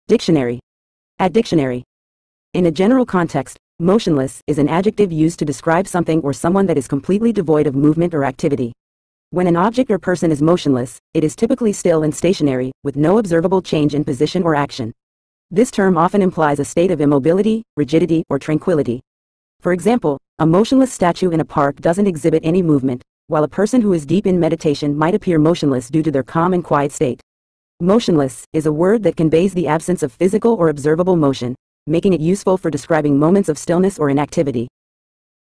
Motionless Meaning In English
In a general context, "motionless" is an adjective used to describe something or someone that is completely devoid of movement or activity. When an object or person is motionless, it is typically still and stationary, with no observable change in position or action. This term often implies a state of immobility, rigidity, or tranquility. For example, a motionless statue in a park doesn't exhibit any movement, while a person who is deep in meditation might appear motionless due to their calm and quiet state. "Motionless" is a word that conveys the absence of physical or observable motion, making it useful for describing moments of stillness or inactivity.